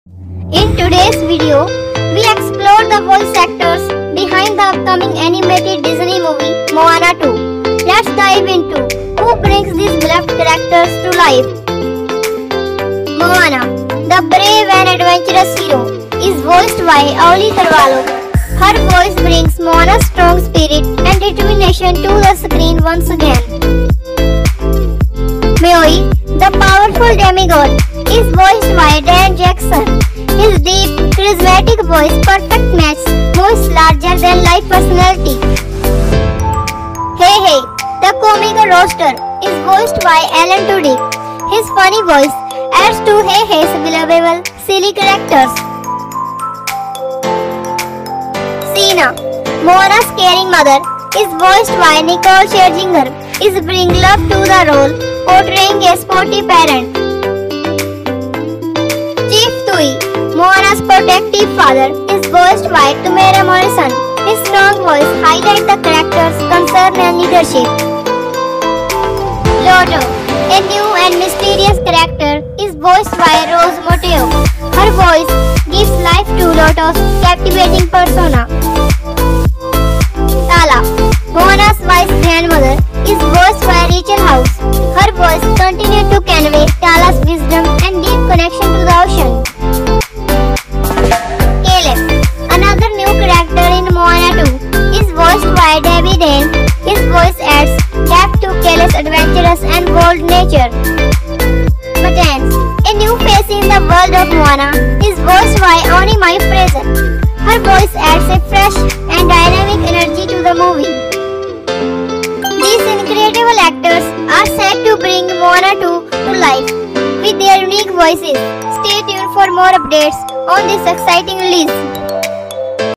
Moana 2: Meet the Voice Actors Behind the Characters
Disney products 🛍🎆🤩👇

Get to know the talented voice actors bringing your favorite characters to life in Moana 2! Discover who’s returning and who’s new in this exciting sequel.

Music 🎶🎵🤩🎆
Musician: @iksonmusic